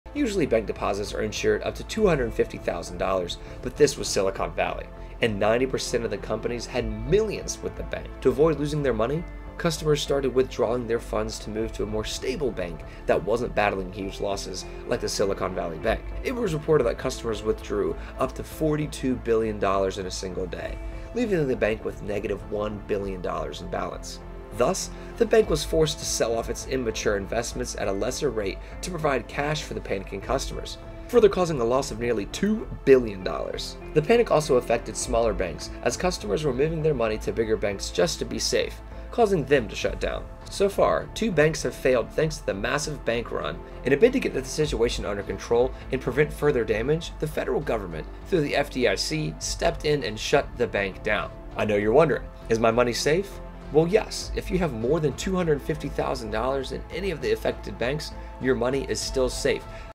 Silicon Valley Bank Collapse - How Does it Affect You?
The recent collapse of Silicon Valley Bank has left many investors wondering what the repercussions will be for them. In this video, I discuss how the bank's failure affects us and offer advice on protecting your investments in light of this news.

for latest in Tech, Finance, and Health!